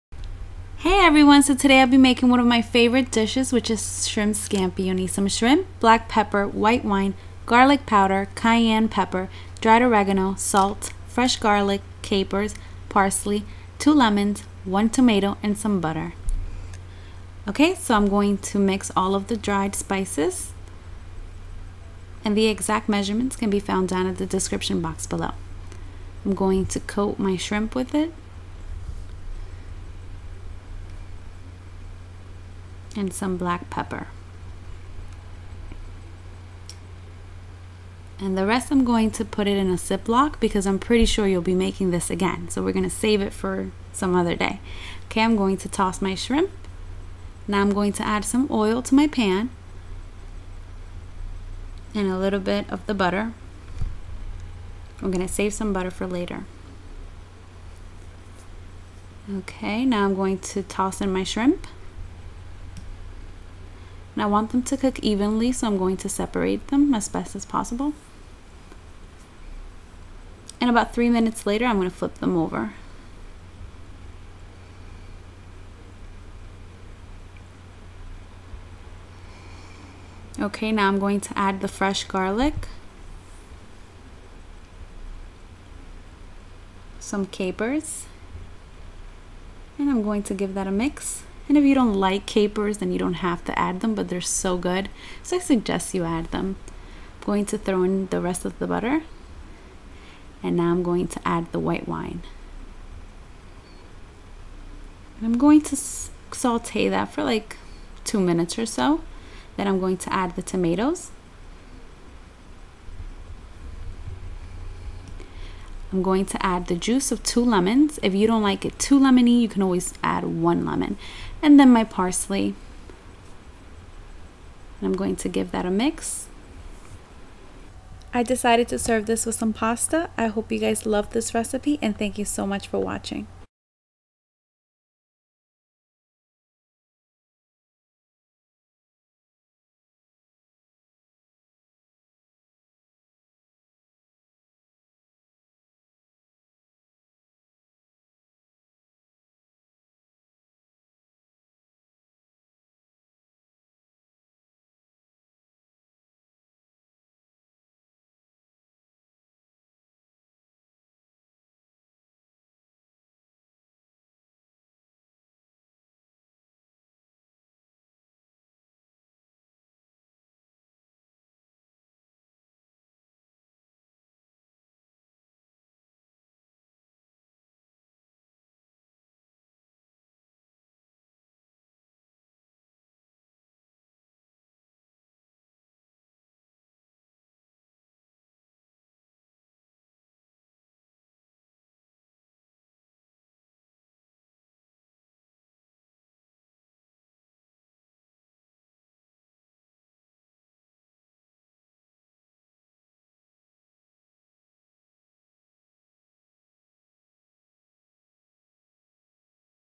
Cajun Shrimp Scampi - Cooked by Julie - Episode 4
Recipe:

1 lb shrimp
4 garlic cloves chopped finely
2 lemons
1 tomato
capers
1/2 cup of white wine
2 tbs unsalted butter
olive oil

Mix together and only add 1 1/2 tsp:
2 tsp garlic powder
2 tsp black pepper
2tsp salt
1 tsp cayenne pepper
1 tsp dried oregano